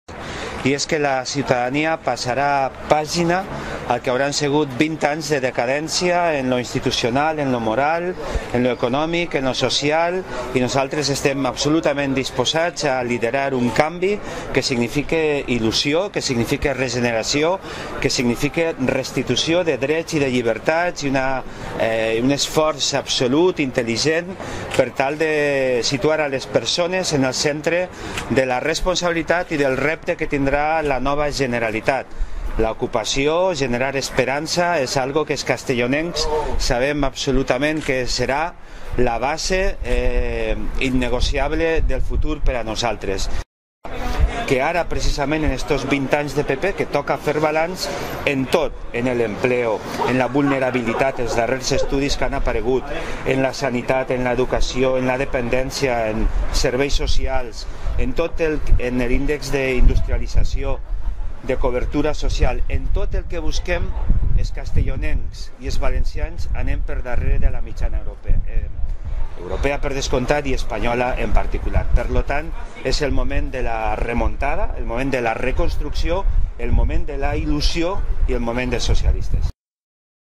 Colomer: "Es el momento de la remontada, de la reconstrucción y de la ilusión"
El secretario general provincial del PSPV y candidato de la lista autonómica por Castellón, Francesc Colomer, ha afirmado hoy que “es el momento de la remontada, de la reconstrucción, el de la ilusión y el momento de los socialistas”. Así de contundente se ha mostrado Colomer tras la presentación de la candidatura de Castellón a Les Corts en la Junta Electoral. Colomer ha estado acompañado por otros miembros de la lista la cual está conformada también por personas independientes que se han querido unir al proyecto de los socialistas para “protagonizar el cambio el próximo 24 de mayo”. Colomer ha señalado que “los socialistas venimos a lo que ya es un ritual en la democracia, presentar nuestras credenciales y nuestra voluntad de participar en estas elecciones”. En este sentido, el también portavoz socialista en la Diputación ha destacado que “vamos a hacer posible algo que estamos convencidos va a pasar, y es que la ciudadanía pasará página a lo que habrán sido 20 años de decadencia en lo institucional, en lo moral, en lo económico y en lo social”. “Estamos dispuestos a liderar un cambio que signifique ilusión, regeneración, restitución de derechos y libertades y un esfuerzo para situar a las personas en el centro de las intenciones y razón de ser del gobierno”, incide.